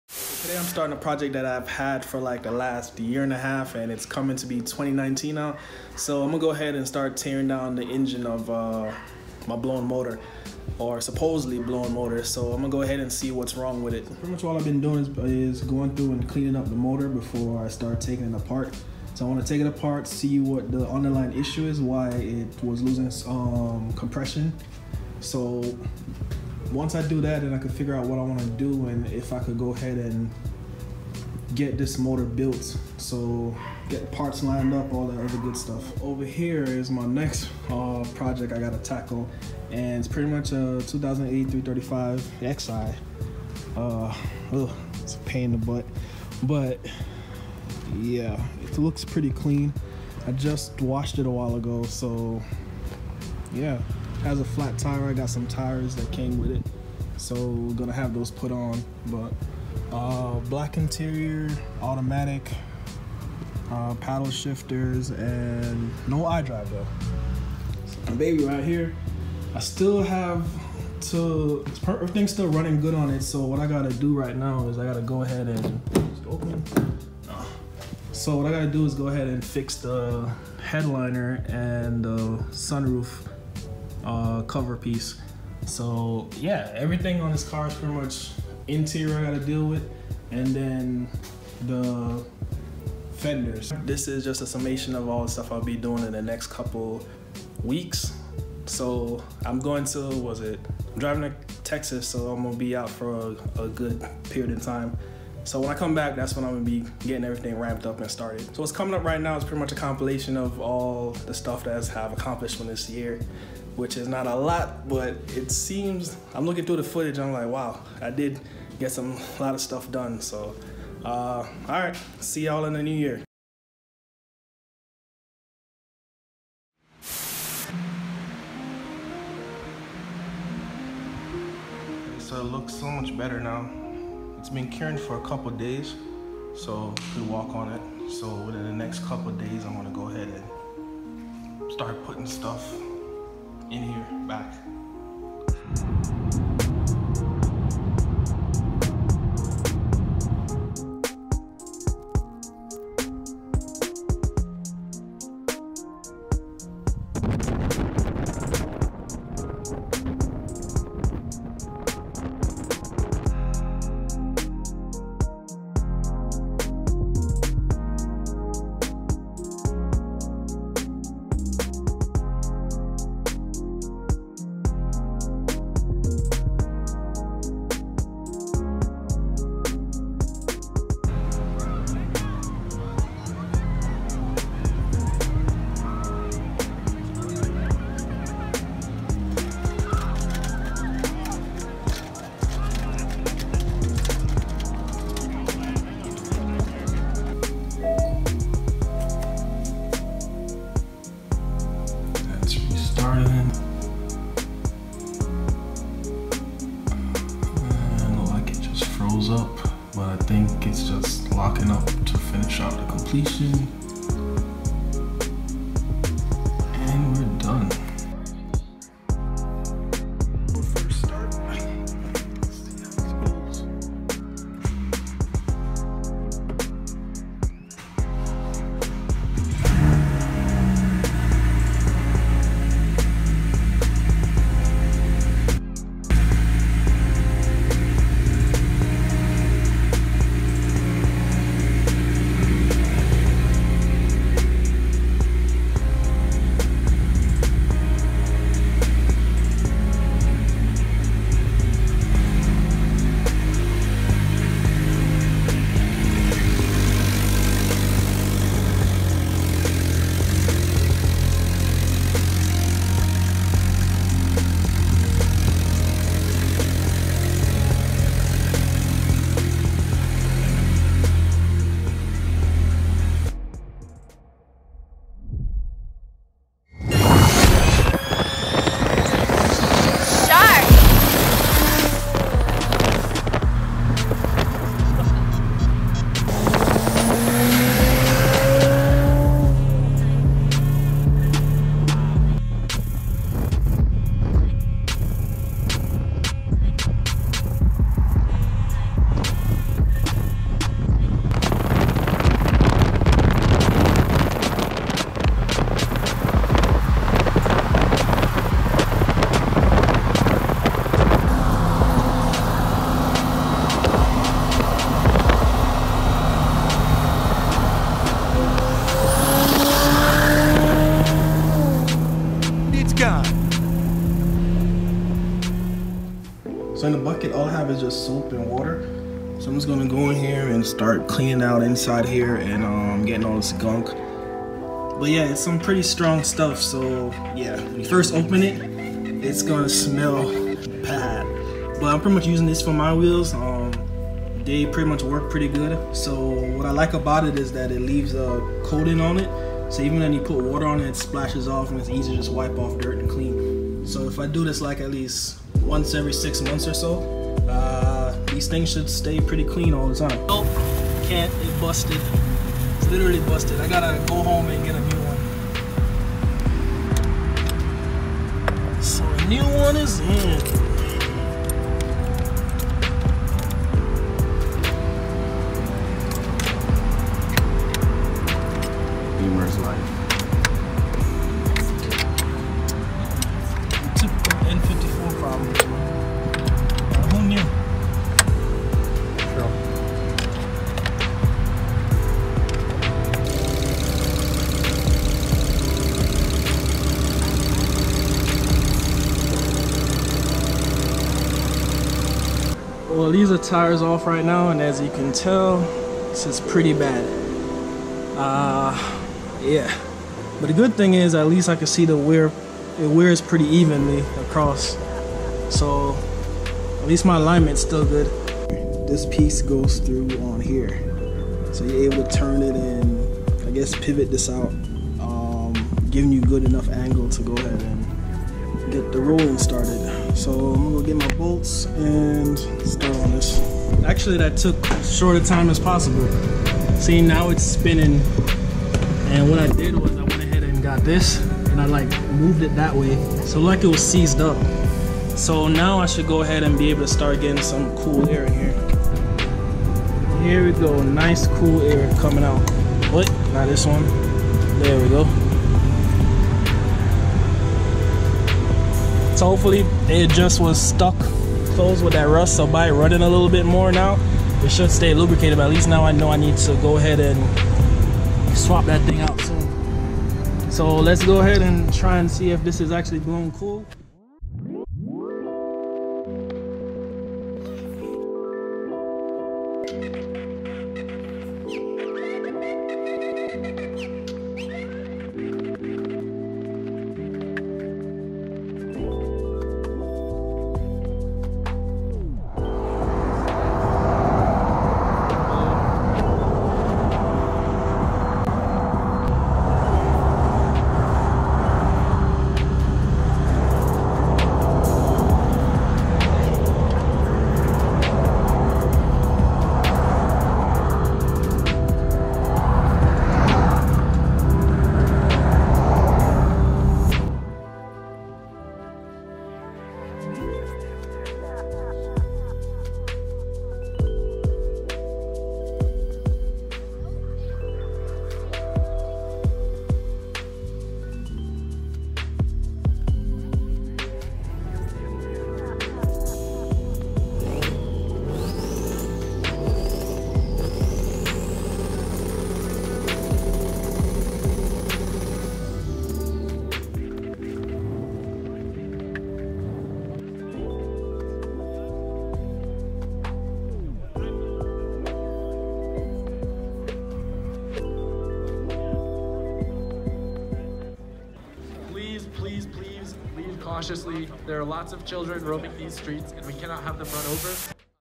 New Project Announcment & 2018 Wrap Up
First, I want to say a huge thank you for your amazing support - for watching, sharing, liking, our videos, and for your positivity and encouragement throughout the year.

This year has been one big whirlwind in which I had limited time to work on anything. The good news is 2019 I have more free time and alot of projects to complete.

The first project is to disassemble my old n54 engine and find the cause of the cylinder 3 no compression. Then I have a 335i xi that needs to be revived from the dead. I need to access everything that is wrong with it and order the needed parts.

Have a Happy New Year !!